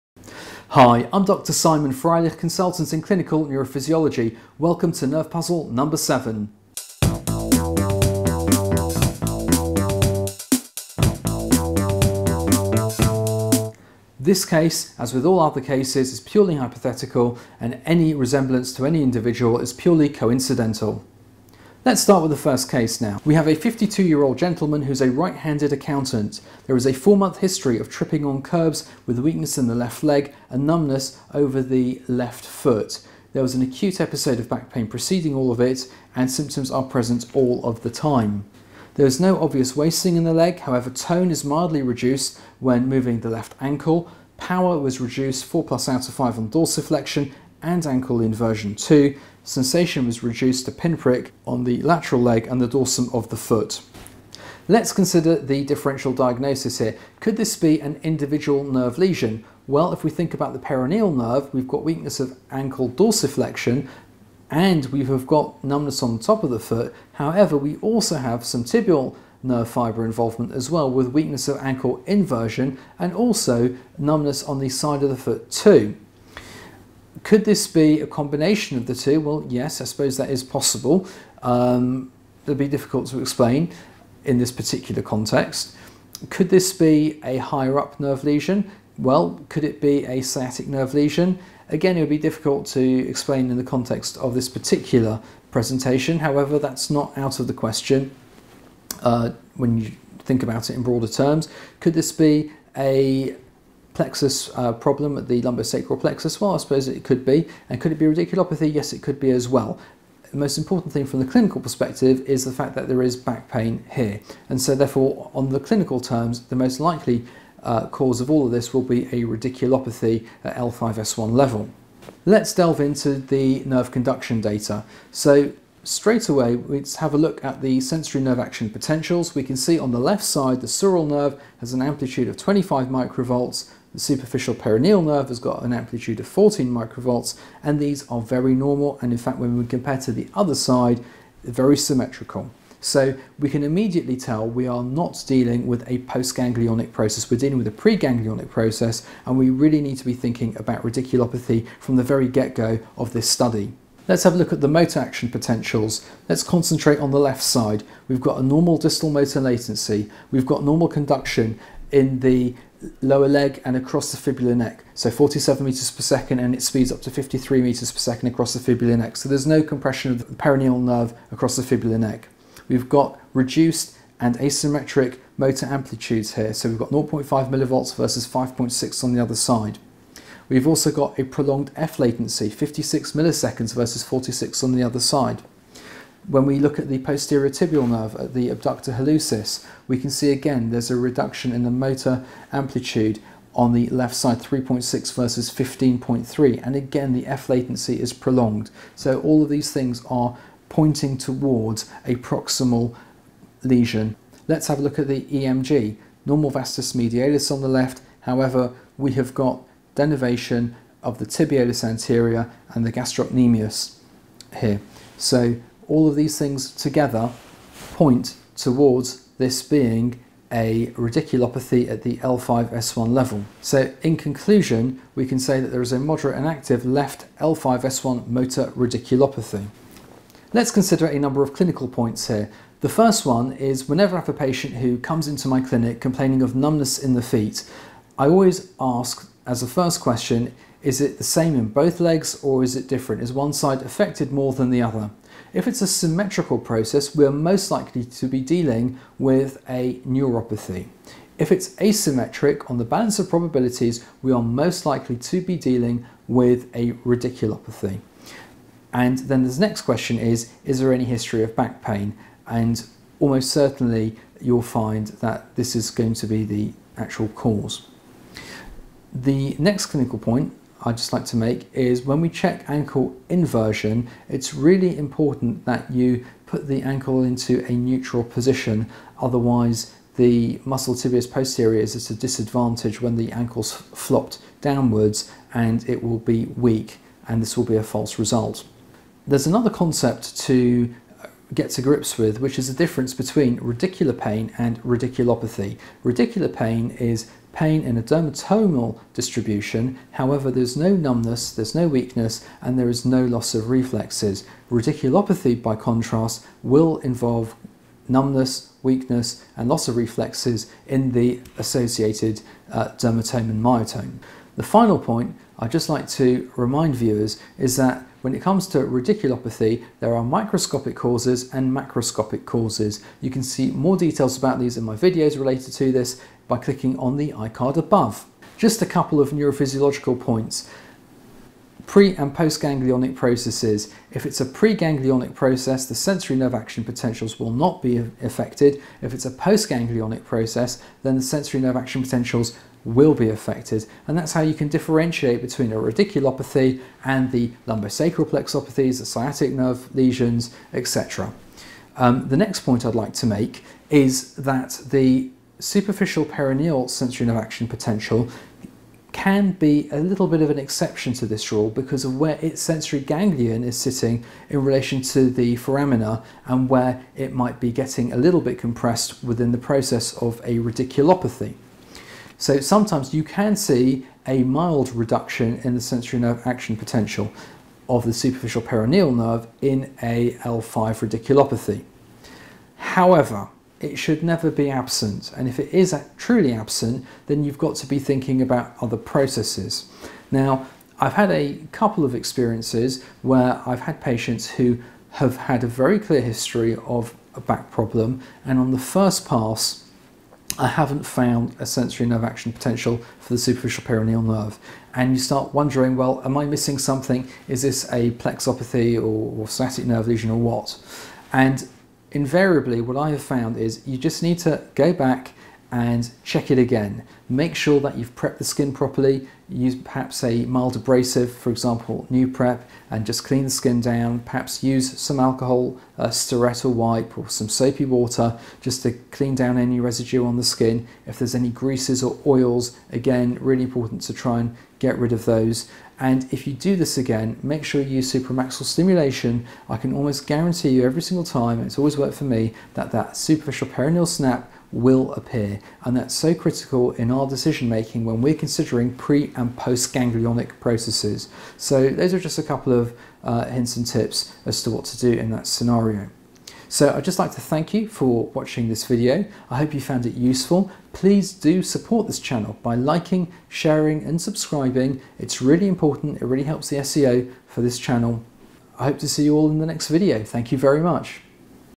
Nerve puzzle 7 - Back up - the neurophysiology assessment of sciatica
This case is one of a lumbar radiculopathy at L5 level. In this video I explain how we differentiate this condition from post-ganglionic processes such as a Lumbo-Saccral Plexopathy, Sciatic Nerve Lesion or combined Peroneal and Tibial Nerve lesions. I also have a few tips and tricks regarding the superficial peroneal nerve when it's not initially apparent.